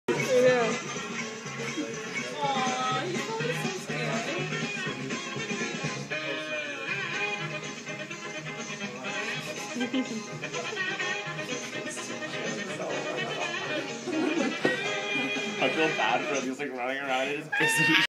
Hamster in a wok wok inception
Watching a hamster in a bowl while watching hamster in a wok